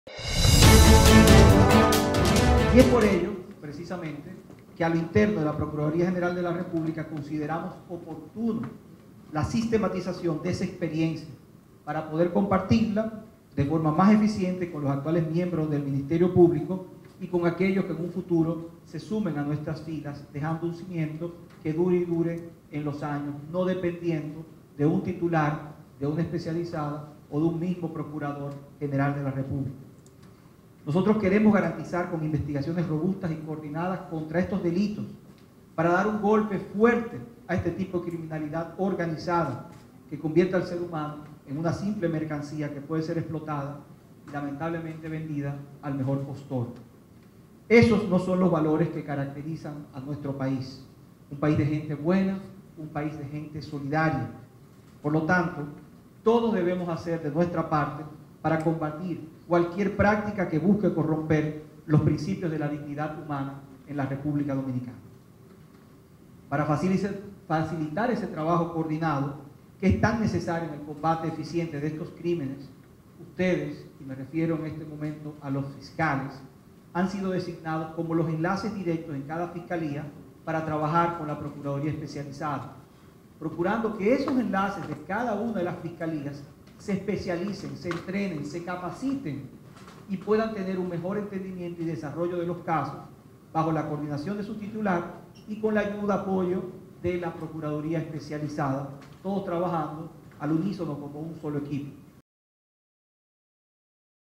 PGR presenta lineamientos operativos contra la trata y tráfico de personas
La Procuraduria General de la Reública (PGR), presentó este lunes los lineamientos contra la trata de personas y el tráfico ilegal de migrantes.